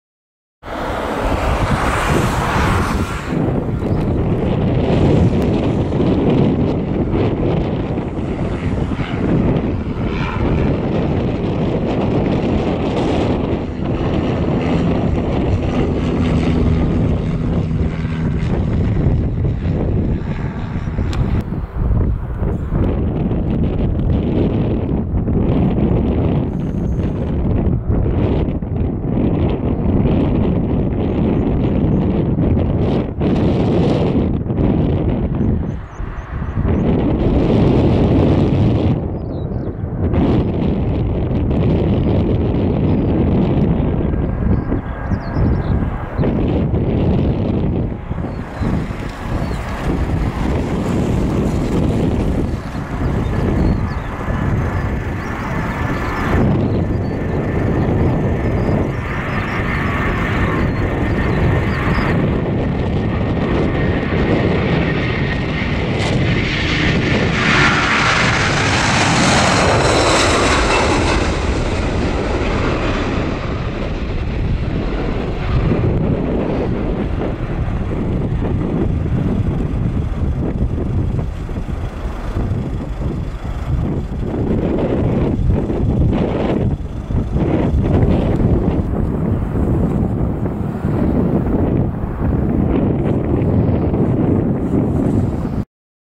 [RARE] Sky Gates 747 - Takeoff at Maastricht Aachen Airport
Here is an very special catch today...The new founded cargo-airline Sky Gates Airlines is performing up to 4 flights a week at Maastricht Airport since a few months. It's a sisterairline of the well known Silkway West Airlines. Silkway will also perform 4 flights a week very soon...Great to see how MST is growing. Have a nice day!!!

Plane: Boeing 747-400F
Airline: Sky Gates Airlines
From: Maastricht
To: Baku